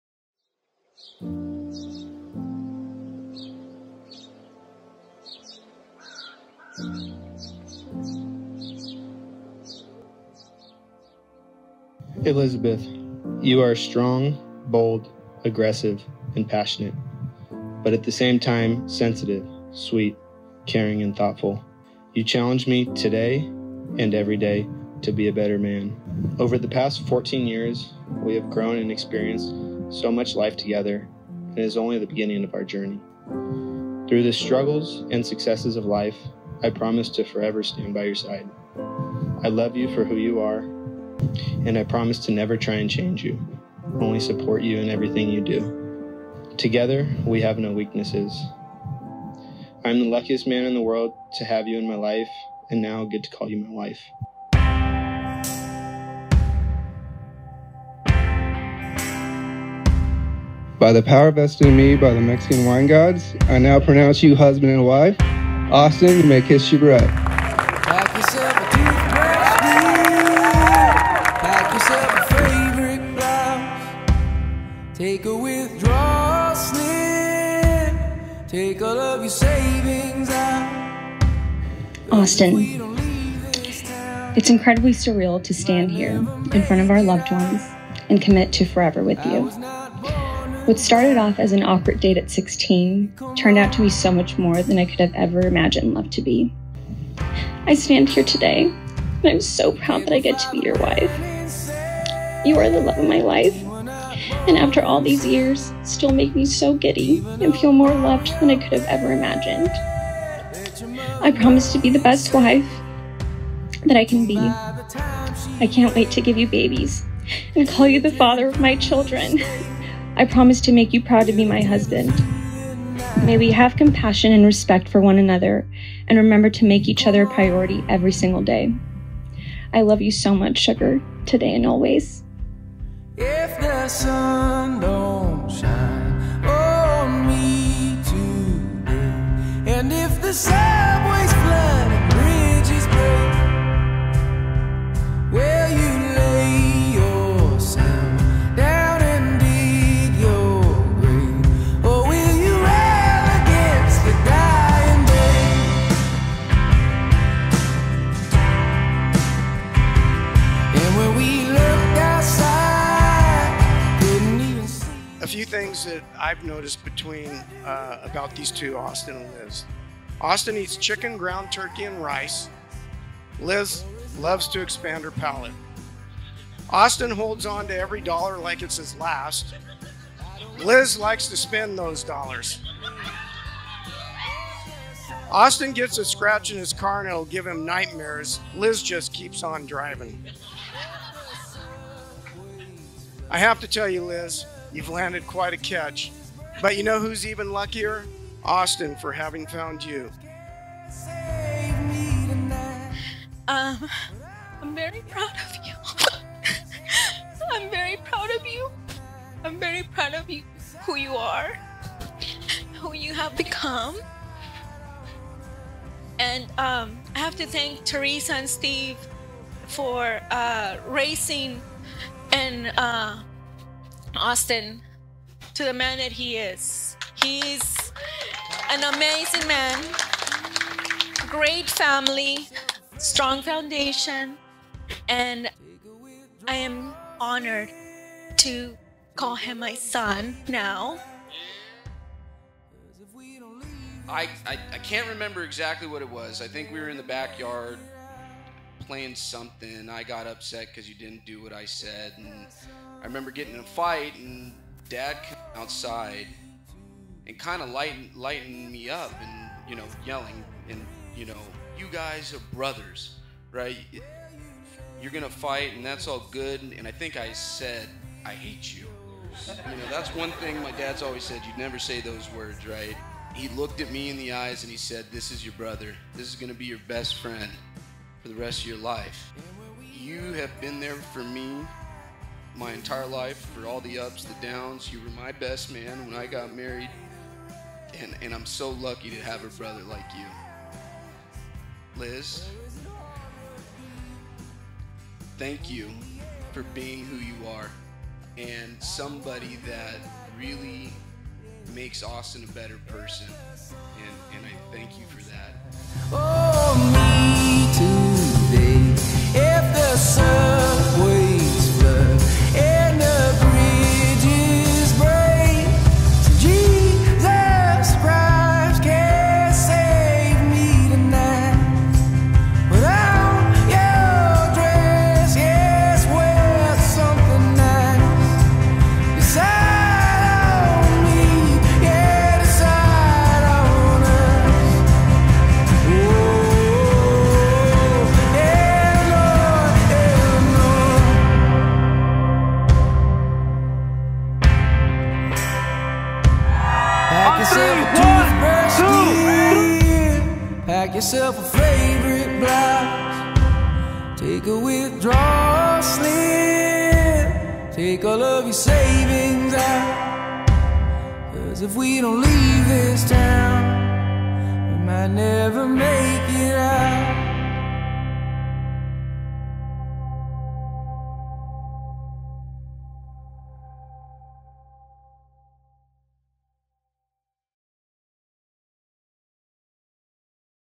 Elizabeth + Austin | Valle de Guadalupe Wedding | Frannes vinicola
Highschool Sweethearts Tie the Knot after 14 Years!
Venue: Frannes vinicola
Wedding Planner: Fany R Eventos
Florist: Shante Eventos